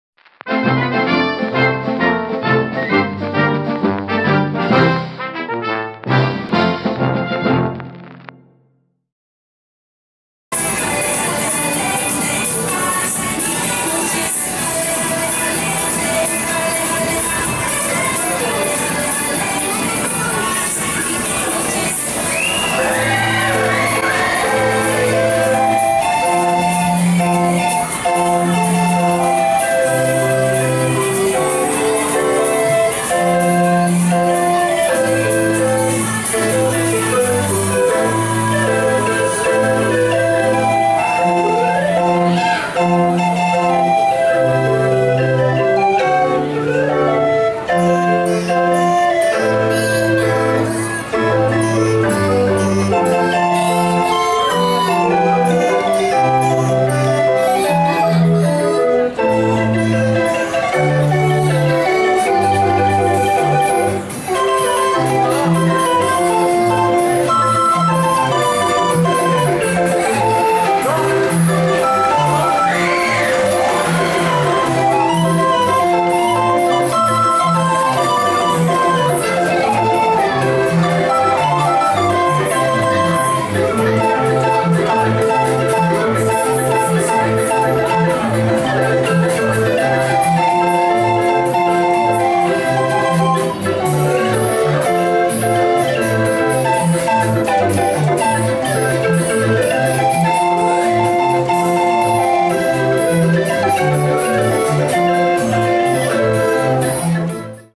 Draaiorgel - Kasteelplein - Plopsaland De Panne - (15-08-2014)
In dit filmpje ziet u de draaiorgel, waar er leuke liedjes op gespeeld worden. Deze orgel speelt bijna een volledige dag.

Veel kijkplezier.